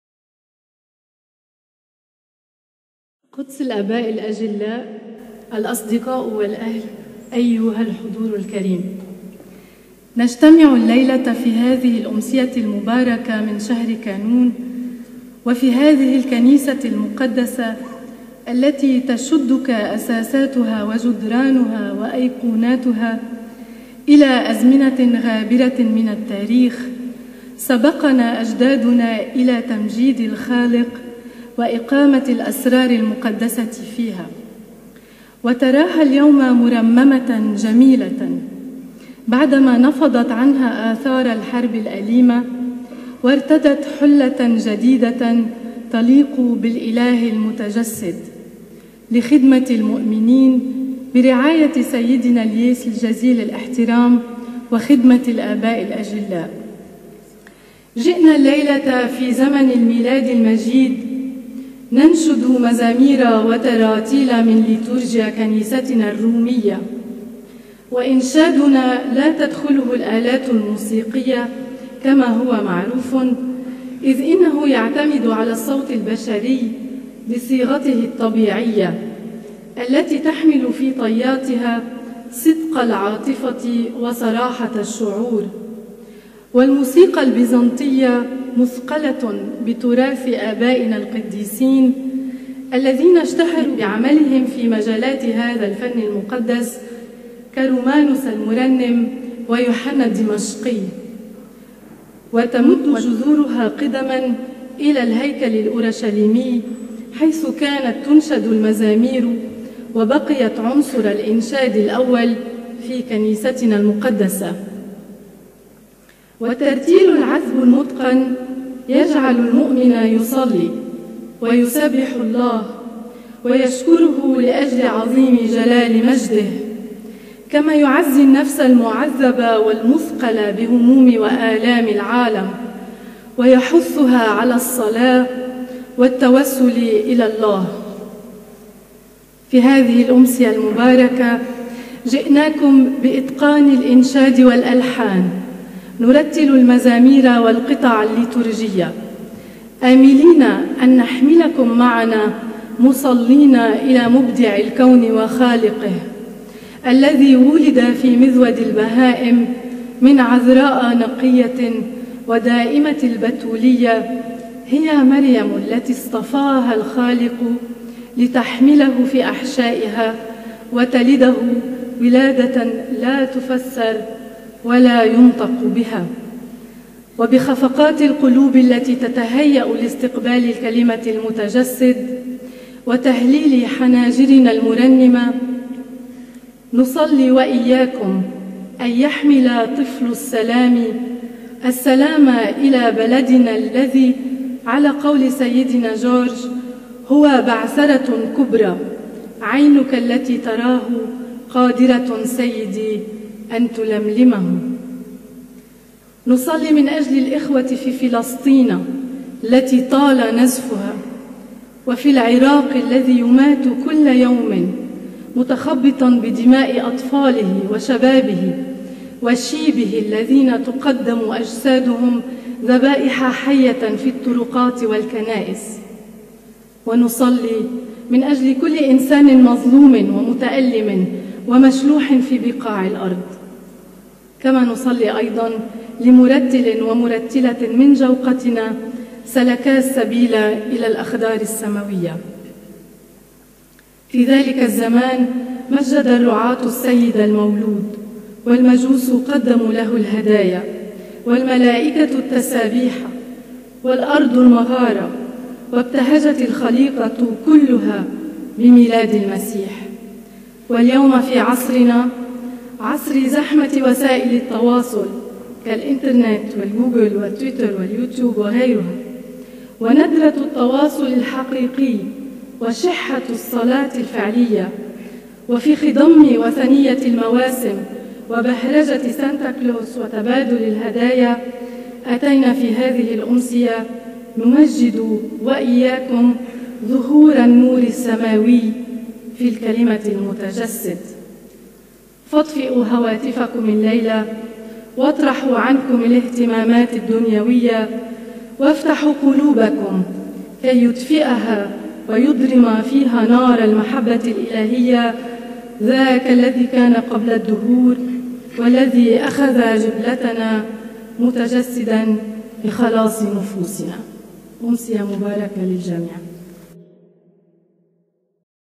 02 SEM address
from sem nativity concert 2010 - with Giorgos Fanaras
SEM address by Nina el Haj Srour